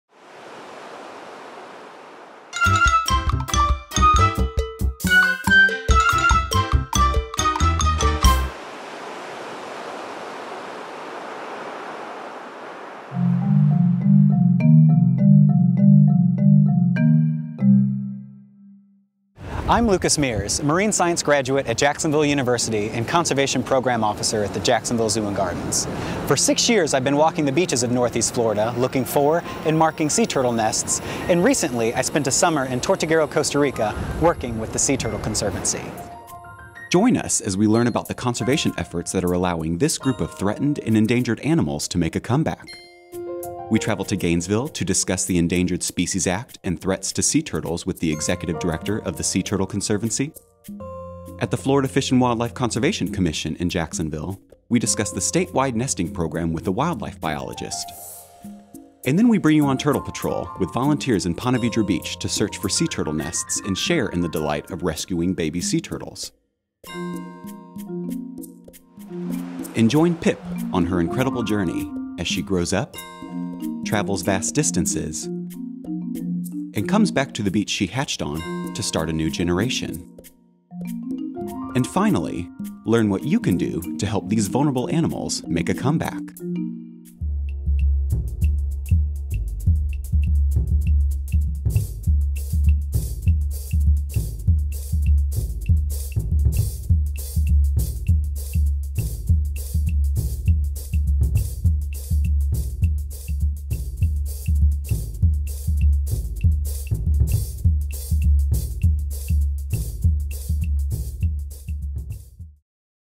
Florida's Endangered Sea Turtles... Part 1 What's to come.
Get a preview of the stories in parts 2-8 of this Florida Sea Turtles video series.